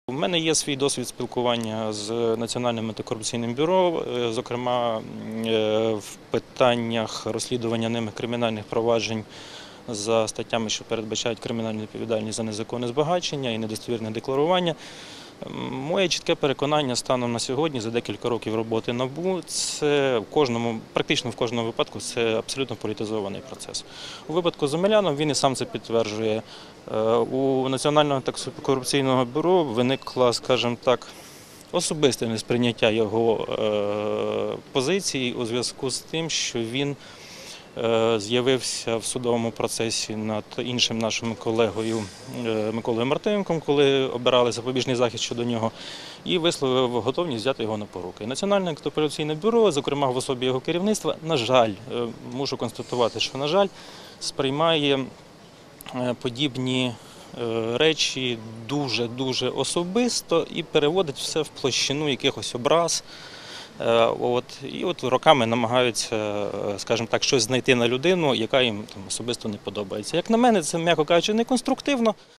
У НАБУ особисте несприйняття Омеляна, - Павло Пинзеник
За кілька років своєї роботи Національне антикорупційне бюро, на жаль, продемонструвало, що більшість ініційованих ним кримінальних проваджень є абсолютно політично вмотивованими.

Про це в інтерв’ю журналістам заявив представник фракції "Народний фронт", в.о. голови Регламентного комітету Верховної Ради Павло Пинзеник, який прийшов до суду підтримати міністра інфраструктури, представника "Народного фронту" Володимира Омеляна.

Він зазначив, що в нього є особистий досвід роботи з представниками Національного антикорупційного бюро, зокрема в питаннях розслідування ними кримінальних проваджень за статтями, що передбачають кримінальну відповідальність за незаконне збагачення, недостовірне декларування тощо.

"Моє чітке переконання: за декілька років роботи НАБУ практично в кожному випадку – це абсолютно політизований процес", - заявив він.

"У випадку з Омеляном він і сам це підтверджує. У НАБУ виникло, скажімо так, особисте несприйняття його позиції у зв’язку з тим, що він з’явився в судовому процесі над іншим нашим колегою Миколою Мартиненком, коли обирався запобіжний захід щодо нього, і висловив готовність взяти його на поруки. Національне антикорупційне бюро в особі його керівництва, на жаль, приймає подібні речі дуже особисто, переводить їх на рівень особистих образ і роками намагається щось знайти на людину, яка їм особисто не подобається. Як на мене, це м’яко кажучи, неконструктивно", - пояснив Павло Пинзеник.

Народний депутат переконаний, що міністр Володимир Омелян доведе свою правоту.

Приєднуйся!